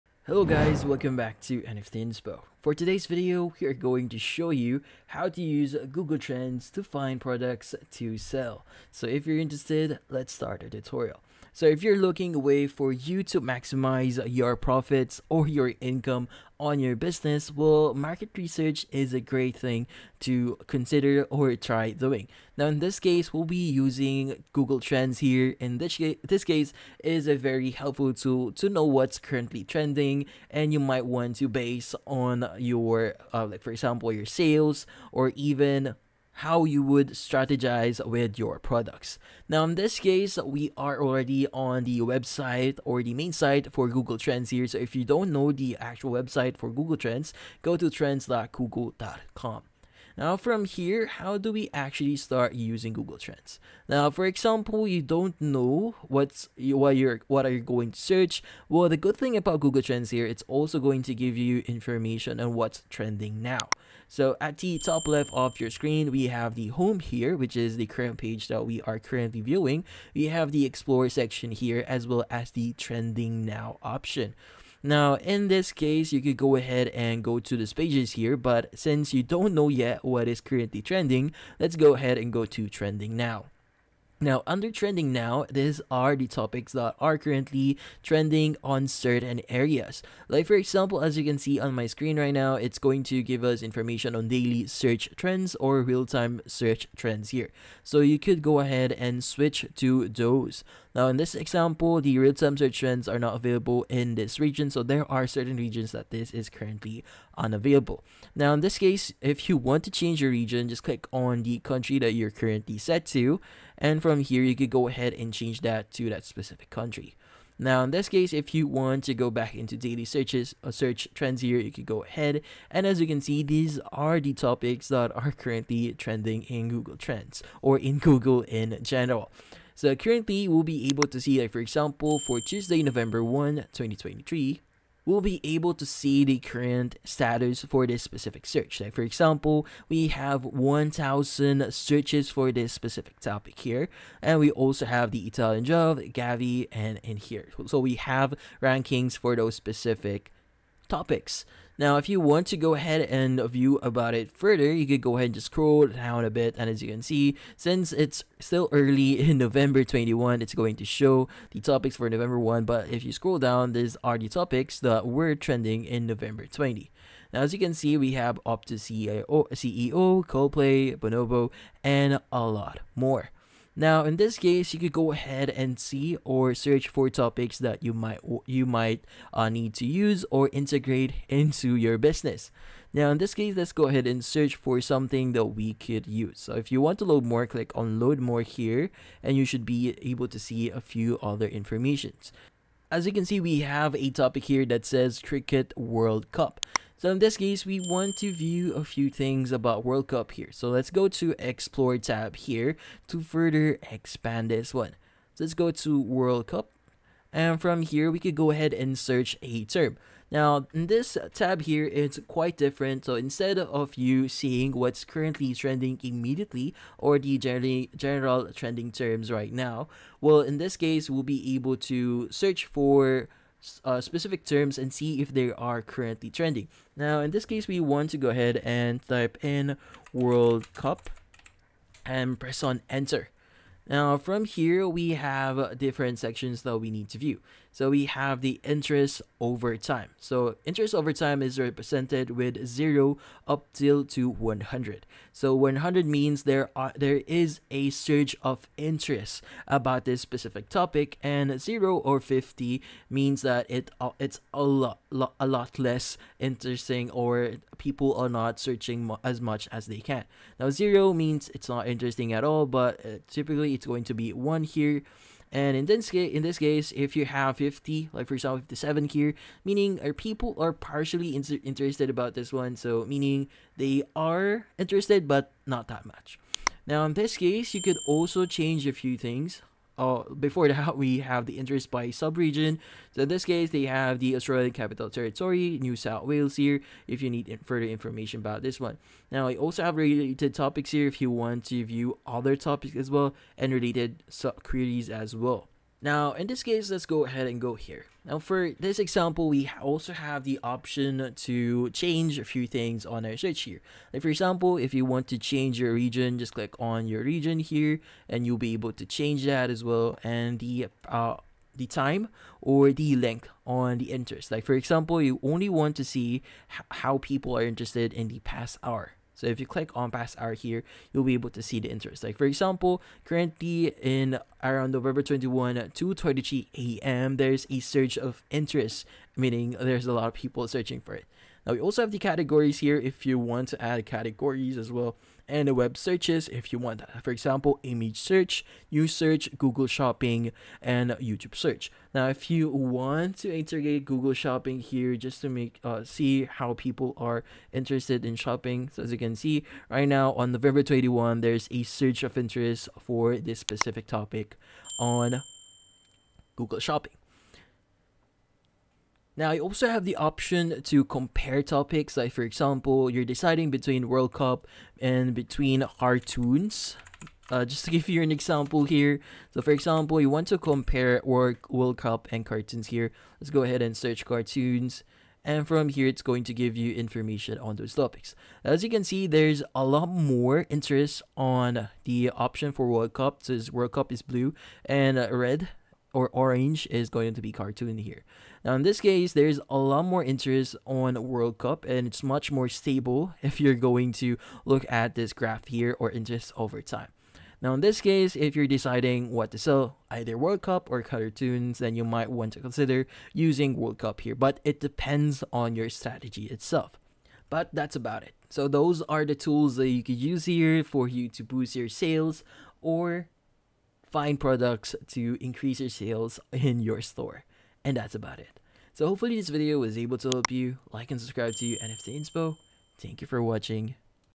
How To Use Google Trends To Find Products To Sell (2025)
Unlock the power of Google Trends to discover trending products for your online business. This tutorial guides you through the process, helping you make informed decisions about what to sell.

Share your experience in the comments below!